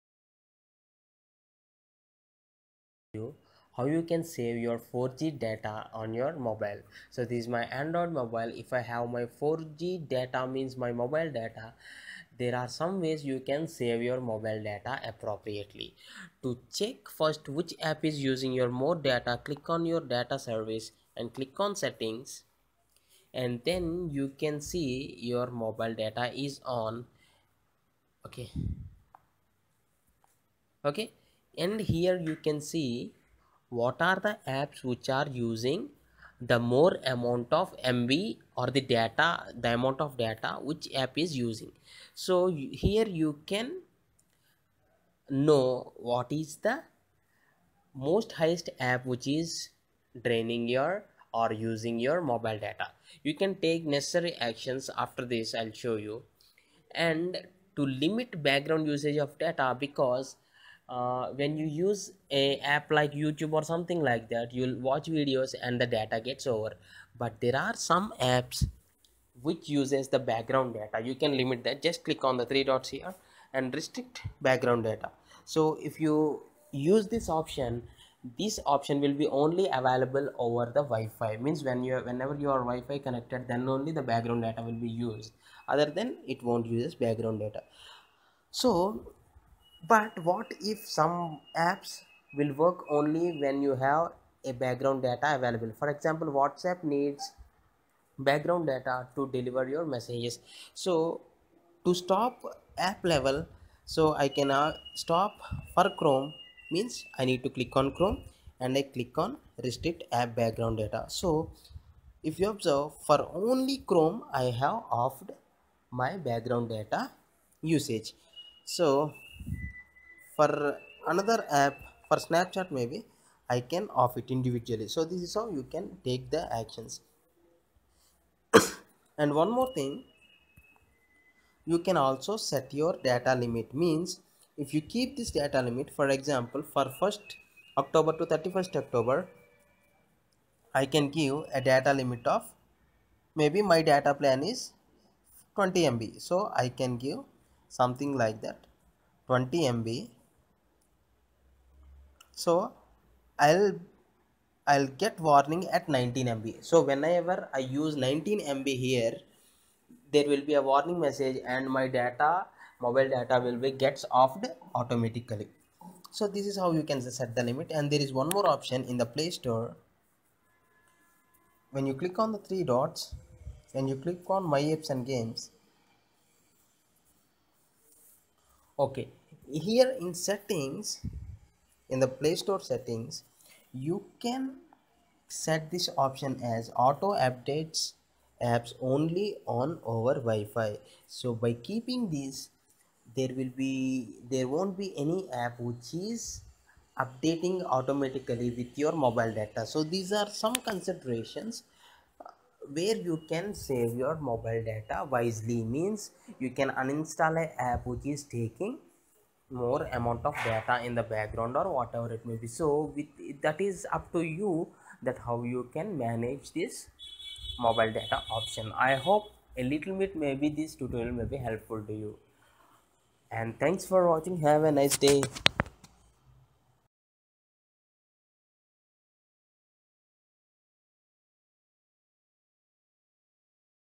how to save 4g data on android
how to save 4g data on android,when you want to restrict background data of 4g data on your android it is simple,by doing this we can save our 4G mobile phone data to a small extent,so there is a bit of chance to save data in android,a few tips were discussed that how to save internet data on android,just follow this tutorial.